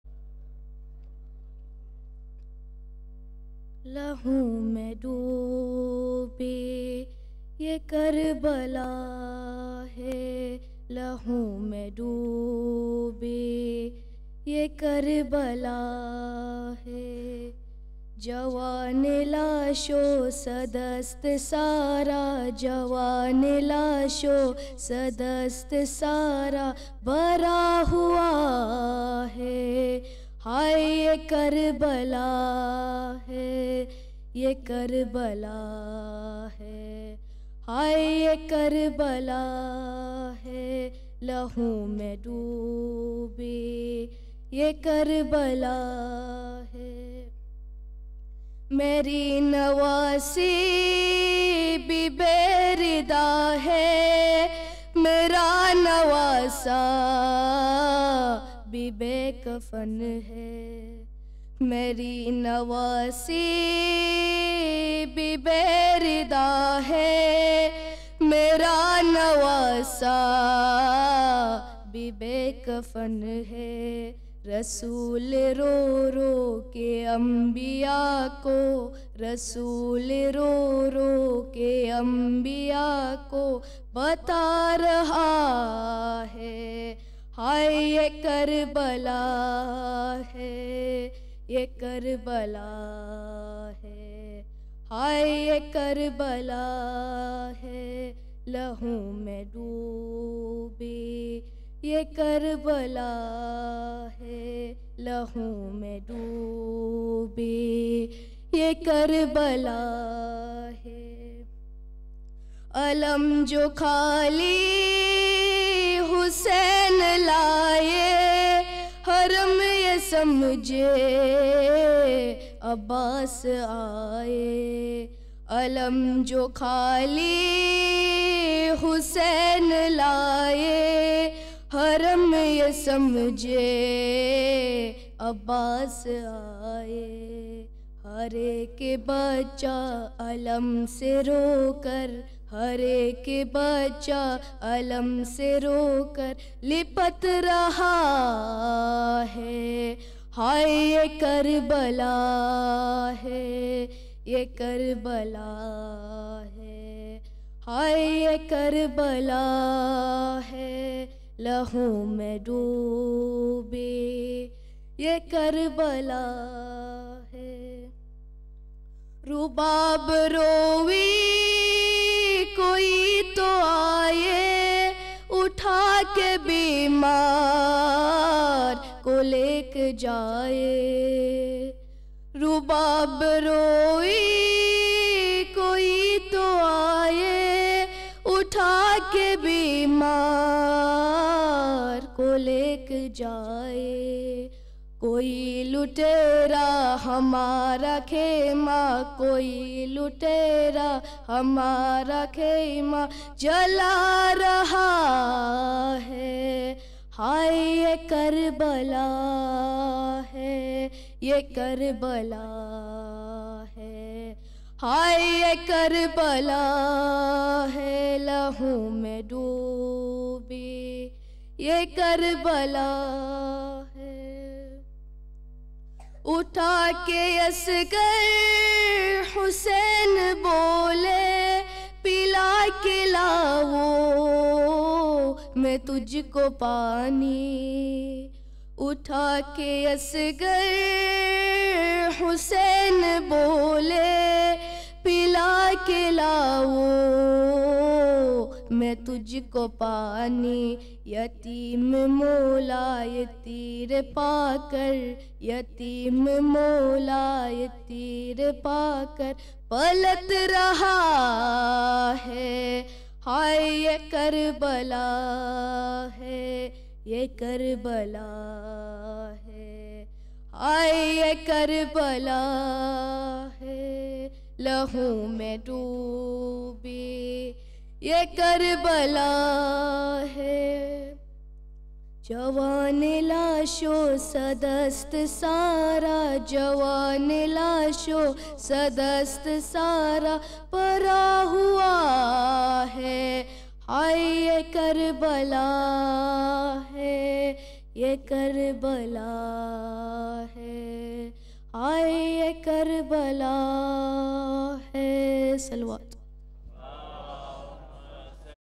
Marsiya 2 - 5th Night of Muharram 1434 / 2012
Marsiya by Mustafa Mawji on 5th Night of Muharram 1434 (19th November 2012)